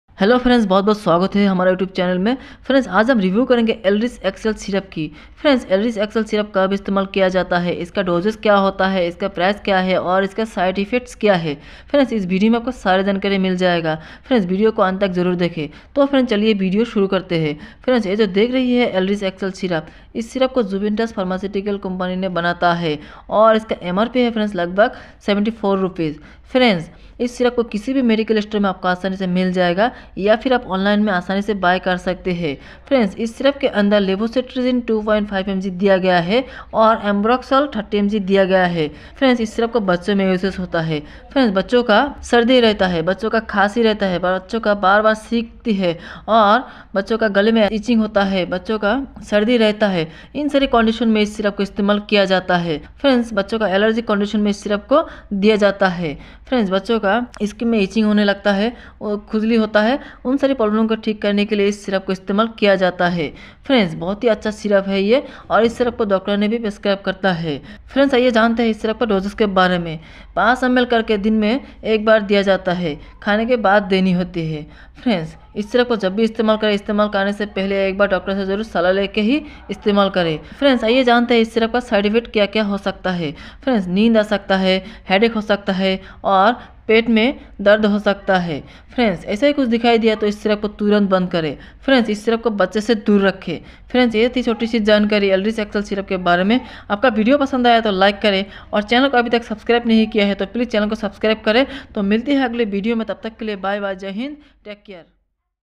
ELRIZ XL Syrup | use Dose Price Side effects | Levoctrizine | Ambroxol | Review in Hindi
Composition
 LEVOCETIRIZINE-2.5MG/5M+AMBROXOL-30MG

Elriz Xl Syrup 30 ml is a respiratory medication used to treat symptoms associated with chronic bronchitis, chronic obstructive pulmonary disease (COPD), allergic rhinitis, and respiratory tract infections, such as cough, sneezing, runny nose, and watery eyes. COPD is a group of lung diseases that mainly include emphysema (shortness of breath) and chronic bronchitis (inflammation of the lining of your bronchial tubes). Allergic rhinitis is an inflammation of the nose caused by an allergen, such as pollen, dust, mould, or flakes of skin from certain animals.

Elriz Xl Syrup 30 ml consists of two medicines: Levocetirizine (anti-histamine) and Ambroxol (mucolytic agent). Levocetirizine is an antihistamine (anti-allergic) drug that reduces the effects of histamine, a chemical substance that causes allergies like runny nose and skin rash. It is used to treat relieve symptoms, such as watery eyes, runny nose, itchy eyes/nose, sneezing, and itching and swelling caused by chronic urticaria (hives). Ambroxol is an 'expectorant' that promotes the secretion of sputum/cough. It is also a 'mucolytic agent' that makes sputum less viscous and makes breathing easier.

Your doctor will decide the dose and duration of the course based on the severity of your disease. Do not stop taking Elriz Xl Syrup 30 ml on your own until the doctor advises you to do so. Like every other medication, Elriz Xl Syrup 30 ml also has common side effects like upset stomach, dryness in the mouth, headache, fatigue, and sleepiness. These side effects, if occurred, generally go away during the treatment. However, if the side effects are persistent, reach out to your doctor.

Let your doctor know if you have any allergic reactions to medicines. It is mandatory to inform your doctor if you have heart, liver, kidney diseases, gastrointestinal disorders, stomach ulcers, prostate enlargement, urinary retention, and diabetes before taking Elriz Xl Syrup 30 ml. Please check with your doctor if you are pregnant or breastfeeding. Avoid alcohol while taking Elriz Xl Syrup 30 ml, since it can worsen the side effects like dizziness and sleepiness. Elriz Xl Syrup 30 ml should be cautiously used in children when advised by the doctor.

Side Effects of Elriz Xl Syrup 30 ml
Upset stomach
Dryness in mouth
Headache
Fatigue
Sleepiness